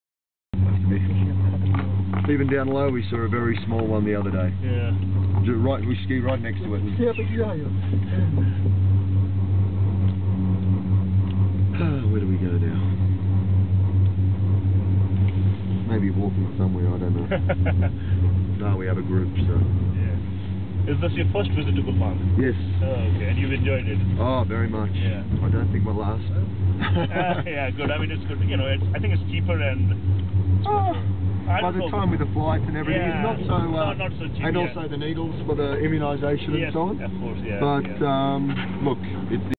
P1030287.MOV
This video was taken while going up to phase 2 of Gondola and you can see the station at the top where you get down to either ski down or you board down on the snow boards. In this video you can see a few snow boarders going down.